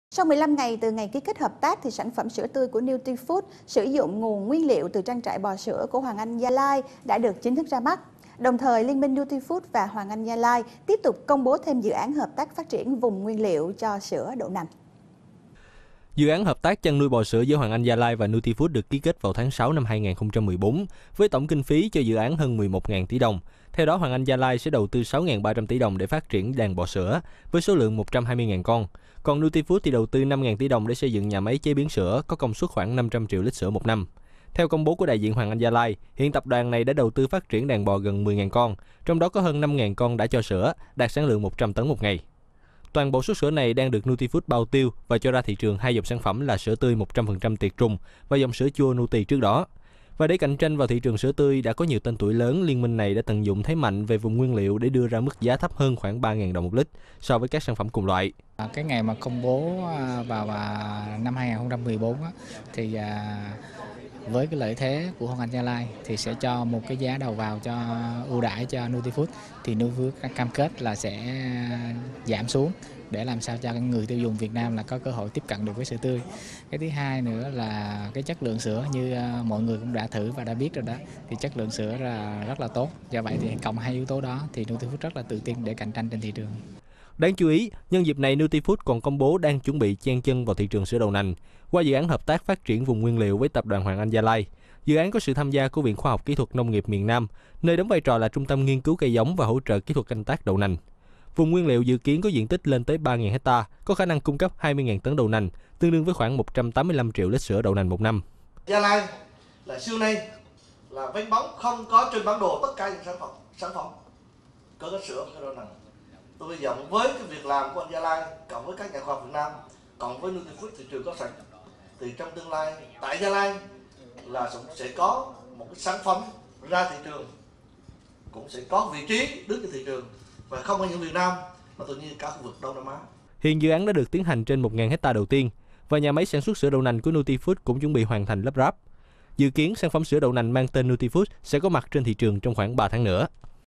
FBNC - Hoàng Anh Gia Lai, Nutifood ra mắt sản phẩm sữa tươi giá rẻ
Sau 15 tháng từ ngày ký kết hợp tác, sản phẩm sữa tươi của Nutifood, sử dụng nguồn nguyên liệu từ trang trại bò sữa của Hoàng Anh Gia Lai đã được chính thức ra mắt. Đồng thời, liên minh Nutifood và Hoàng Anh Gia Lai tiếp tục công bố thêm dự án hợp tác phát triển vùng nguyên liệu cho sữa đậu nành.